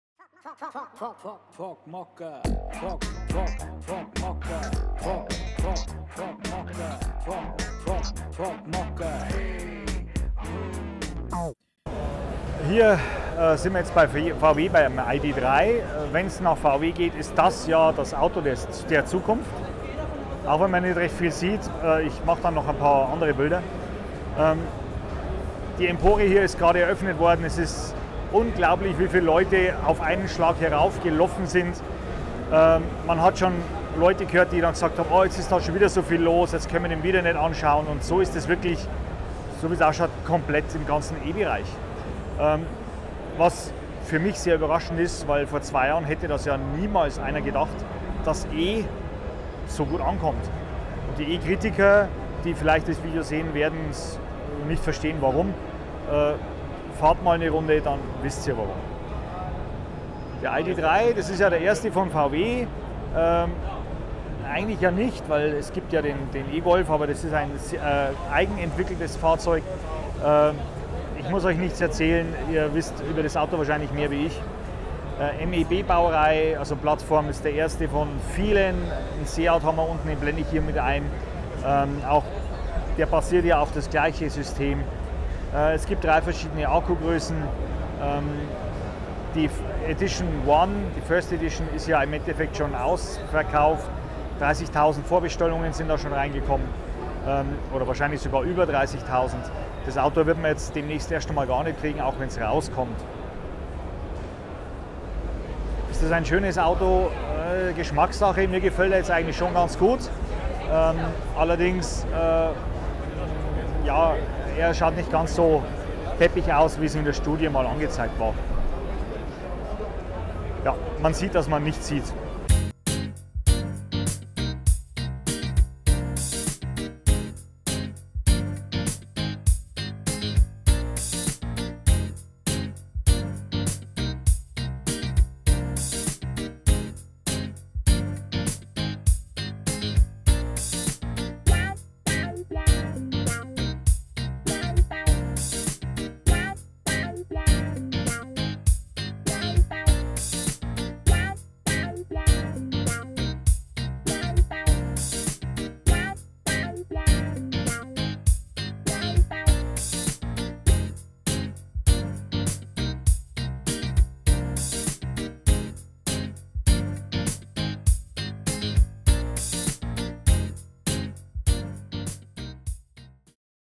IAA 2019 Teil 2 von 8: Volkswagen ID3
Auf der IAA 2019 in Frankfurt wurden endlich einmal bezahlbare E Autos vorgestellt, die man auch kaufen kann.
Eine Videoreihe mit den wichtigsten E Auto Neuheiten die es wirklich gibt.